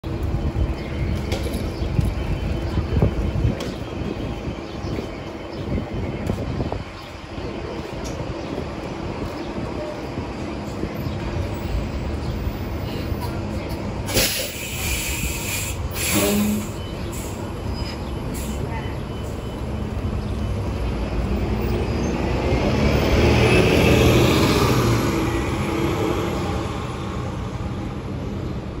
East Harlem bound 2017 New Flyer XD60 #6115 M103 at 3 Ave - East 125 St
Recorded on Thursday June 23, 2022

For some reason a few 2017 XD60s that were transferred from Mother Clara Hale to Tuskegee Airmen depot still have the SBS wrap